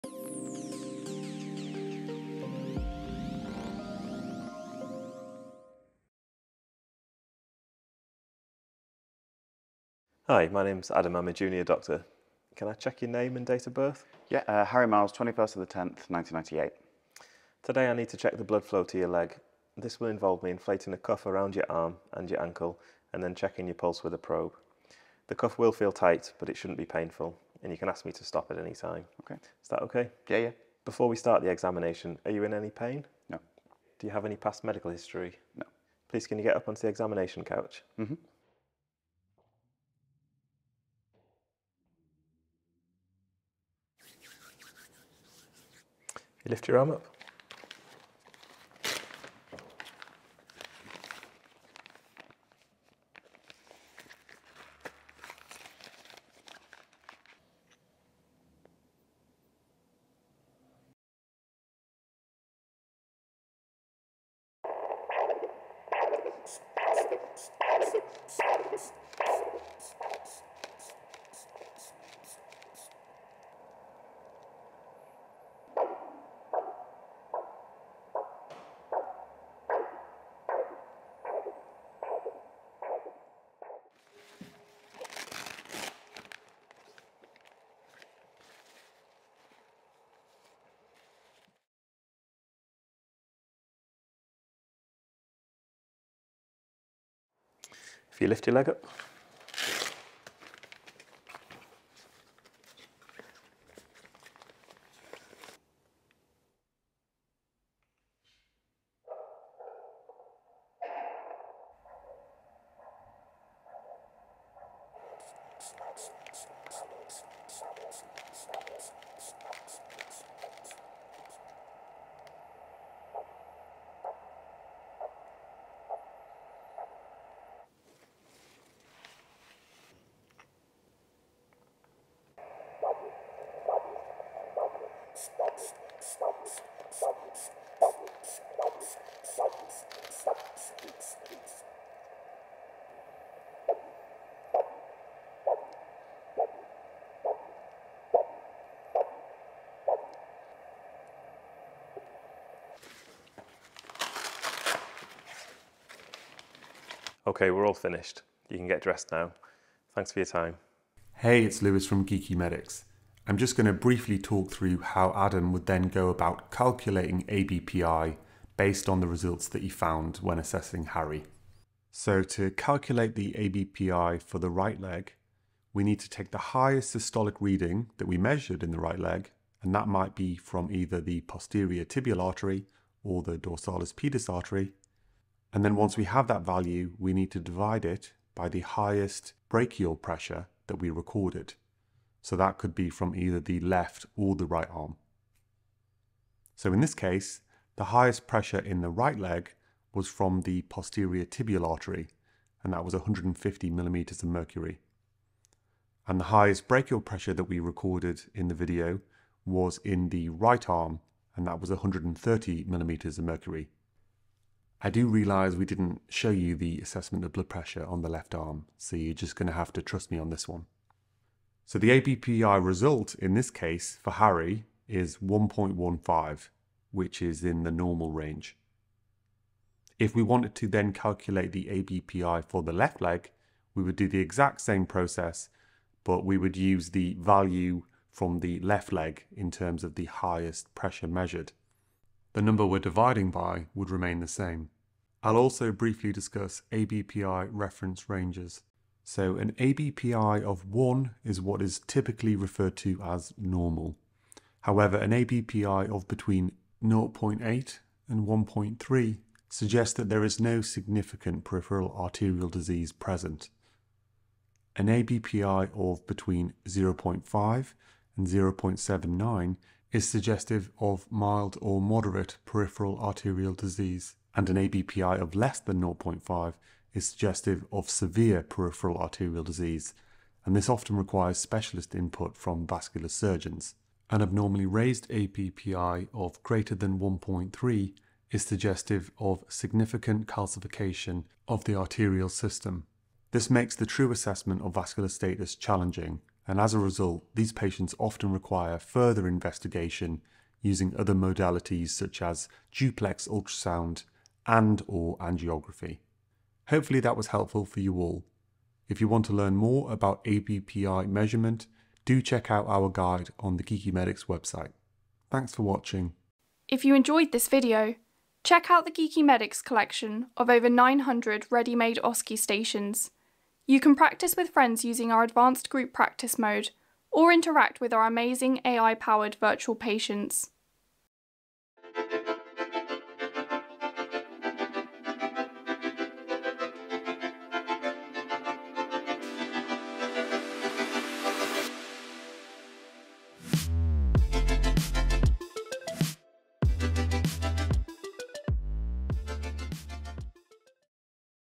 ABPI Measurement - OSCE Guide | Doppler Pulse | UKMLA | CPSA | PLAB 2
This video demonstrates how to perform measurement of ABPI in an OSCE station. The ankle-brachial pressure index (ABPI) is a non-invasive method of assessing peripheral arterial perfusion in the lower limbs. ABPI is a ratio composed of the blood pressure of the upper arm (brachial artery) and the blood pressure of the lower limb (dorsalis pedis and the posterior tibial artery).

Chapters:
00:00 Introduction
00:46 Brachial pressure
01:37 Ankle pressure
01:46 Posterior tibial artery
02:10 Dorsalis pedis artery
02:40 How to calculate ABPI
04:05 ABPI reference ranges